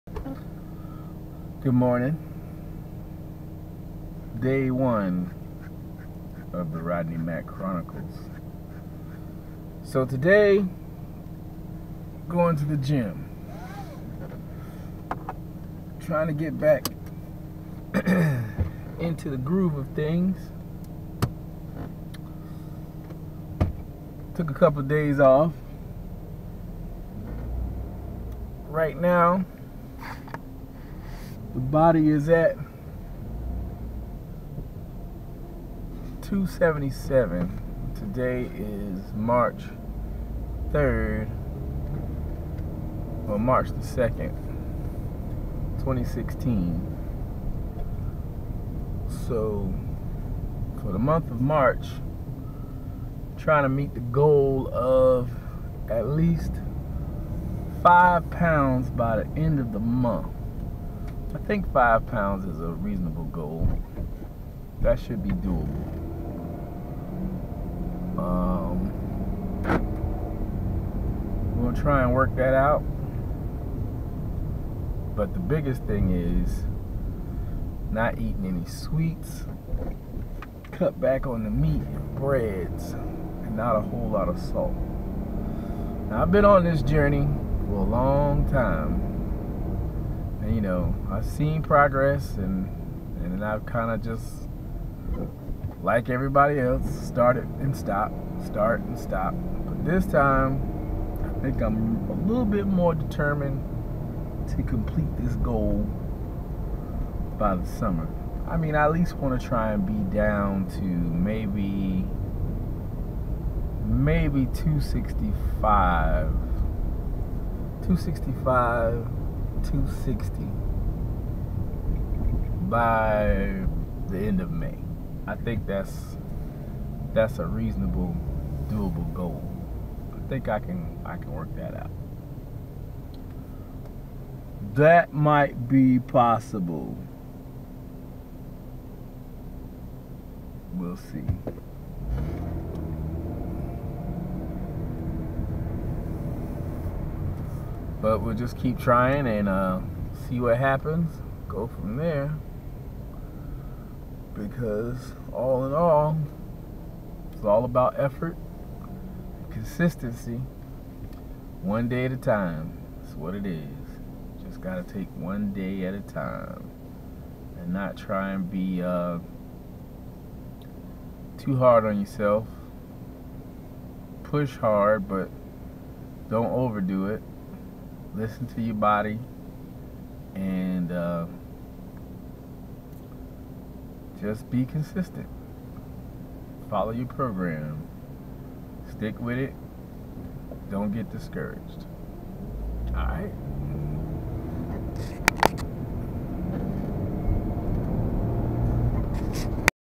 Day 1 "Lets get in shape"
The start of my video journal to document changes 277lbs 3/2/20116